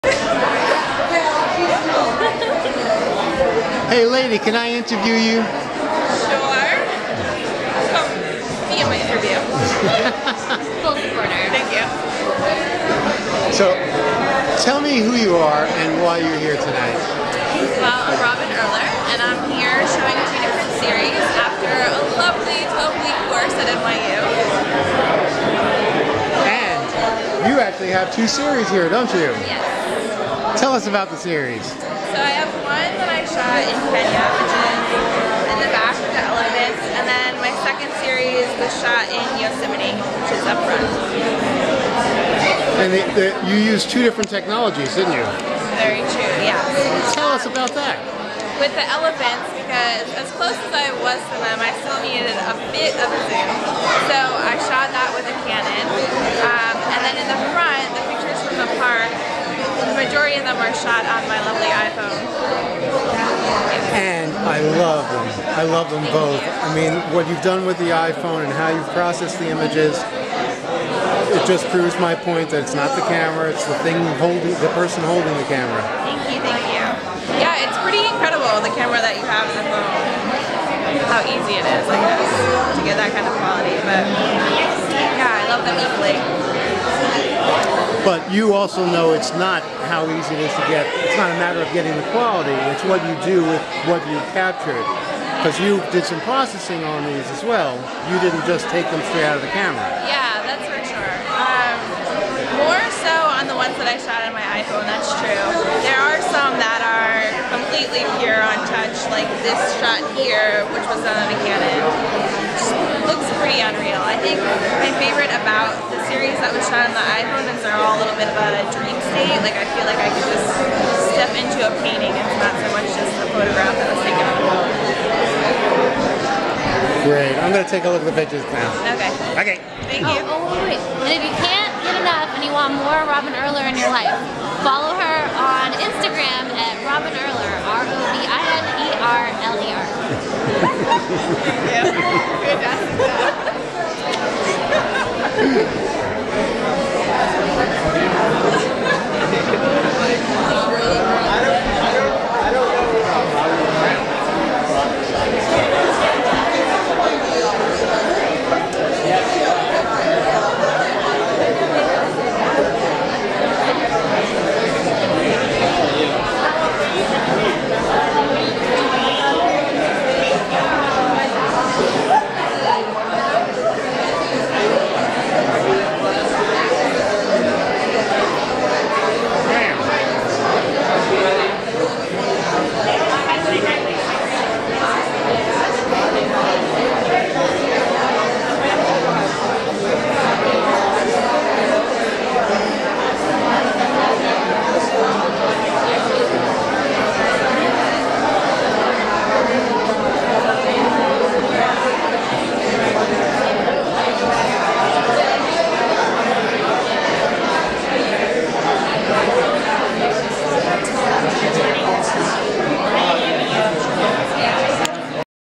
Robin at Soho Photo Gallery
This is from one of the two Spring 2012 shows of my "Photography: From Beginner To Exhibitor" courses (also know as "From Beginning Photographer to Exhibiting Artist" ) I teach at NYU-SCPS, and this "One Nite Floor Show" (opening & reception) occurred Wednesday April 25, 2012 at the Soho Photo Gallery, TriBeCa, NYC, and was the 22nd show in the series. Some of these students had experience in photography, others just took their first pictures 11 weeks ago (when the course started), yet ALL excelled.